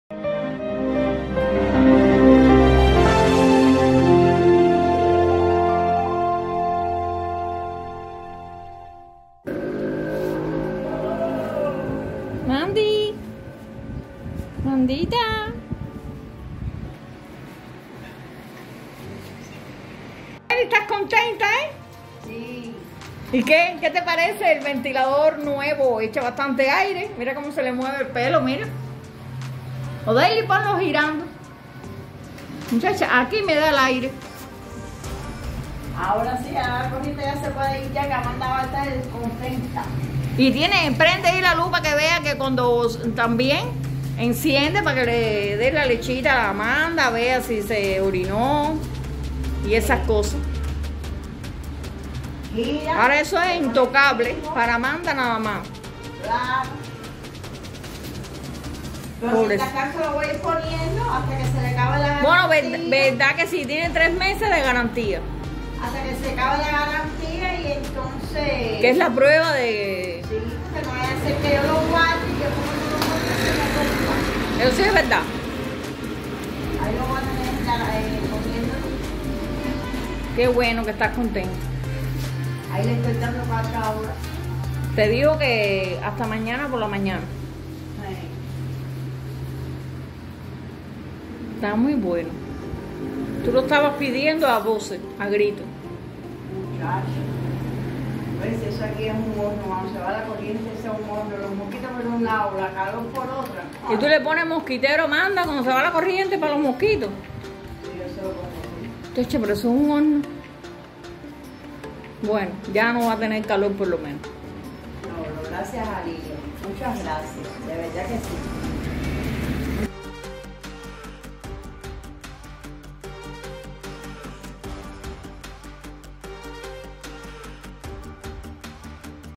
SUSCRIPTORA ENVIA REGALO SORPRES A NIÑA CON DISCAPACIDAD EN CUBA❤️💕😍🥰
What a supercool surprise! A subscriber sent a gift to a little girl with disabilities in Cuba! 🎁 Check out the video to see the amazing surprise and hear more about how the gift came about! Thank you, LILIAM Y PEPE , for making this girl's day! 🎁